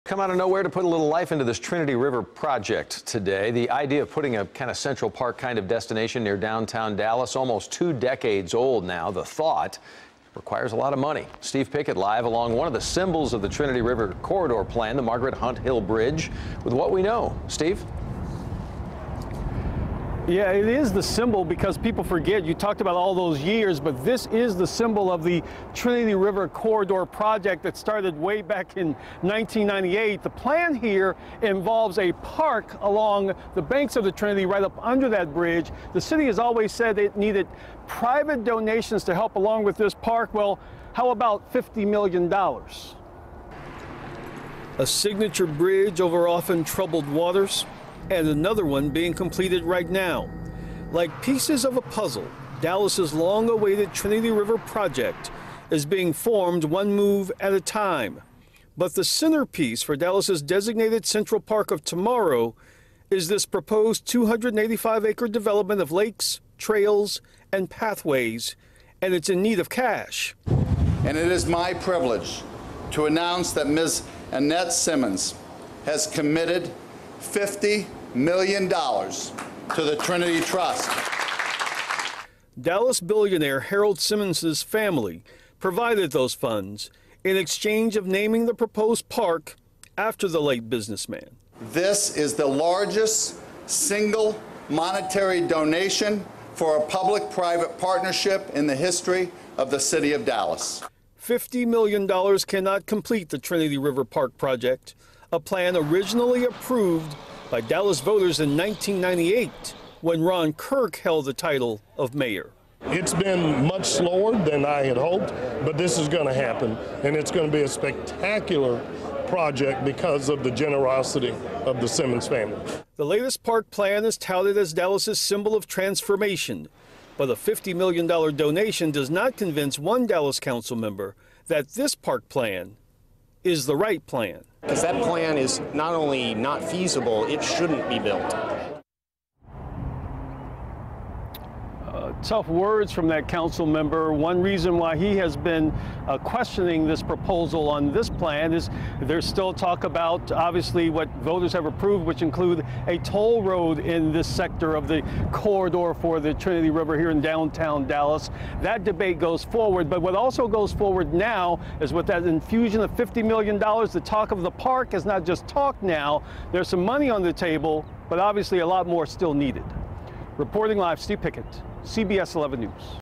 Mayor Rawlings Announces $50 Million Donation For Trinity River Park
Rawlings revealed Monday that the city was given a $50 million donation from Annette Simmons for the new park.